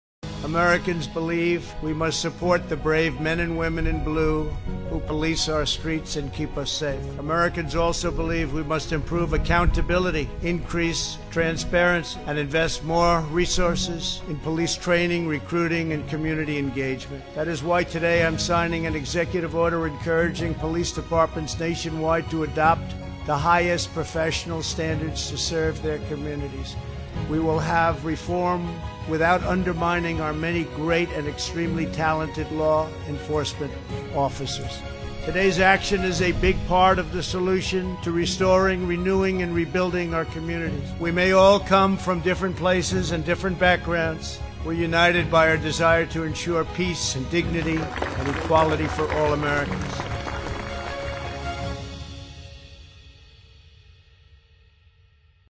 White House Tribute Video on Police Reform
The White House followed up the signing of the executive order on police reform with a moving video tribute of the landmark executive order.